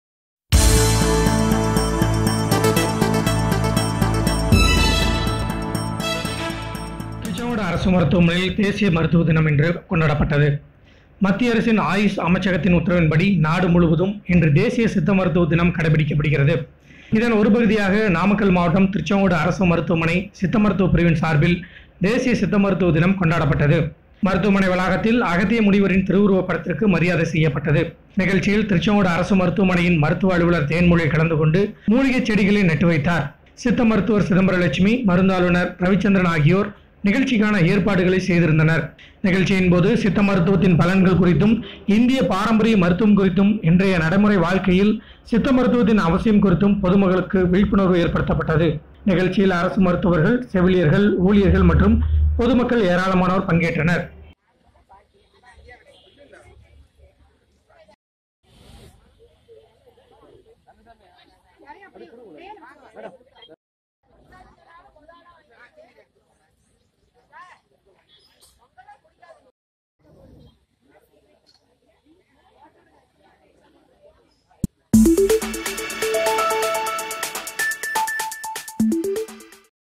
திருச்செங்கோட்டில், தேசிய சித்த மருத்துவ தினம்
திருச்செங்கோடு அரசு மருத்துவமனையில் இன்று தேசிய சித்த மருத்துவ தினம் கொண்டாடப்பட்டது.